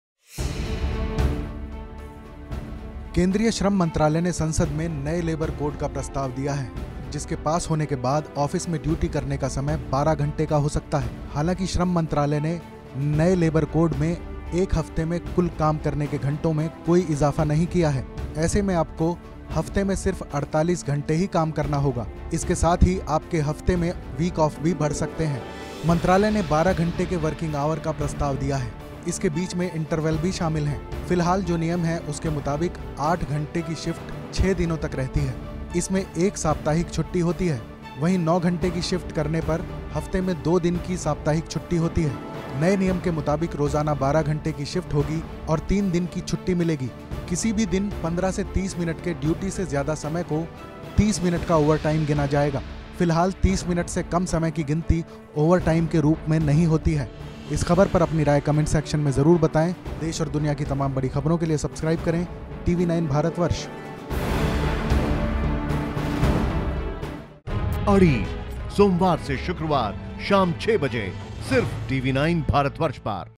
नौकरी करने वालों के लिए 12 घंटे की हो सकती है आपकी Shift, बदलेगा Labour Law में छुट्टी का नियम !
केंद्रीय श्रम मंत्रालय ने संसद में नए लेबर कोड का प्रस्ताव दिया है, जिसके पास होने के बाद ऑफिस में ड्यूटी करने का समय 12 घंटे का हो सकता है. हालांकि श्रम मंत्रालय ने नए labor code में एक हफ्ते में कुल काम के घंटों में कोई इजाफा नहीं किया है. ऐसे में आपको हफ्ते में सिर्फ 48 घंटे ही काम करना होगा. इसके साथ ही आपके हफ्ते में Week off भी बढ़ सकते है.. मिनिस्‍ट्री ने 12 घंटे के वर्किंग आवर का प्रस्ताव दिया है. इसके बीच में Interval भी शामिल हैं. फिलहाल जो नियम हैं, उसके मुताबिक 8 घंटे की शिफ्ट छह दिनों तक रहती है. इसमें एक साप्ताहिक छुट्टी होती है. वहीं 9 घंटे की शिफ्ट करने पर हफ्ते में दो दिन की साप्ताहिक छुट्टी होती है. नए नियम के मुताबिक रोजाना 12 घंटे की शिफ्ट होगी और तीन दिन की छुट्टी मिलेगी. किसी भी दिन 15 से 30 मिनट के ड्यूटी से ज्यादा समय को 30 मिनट का ओवरटाइम गिना जाएगा. फ़िलहाल 30 मिनट से कम समय की गिनती ओवरटाइम के रूप में नहीं की जाती है.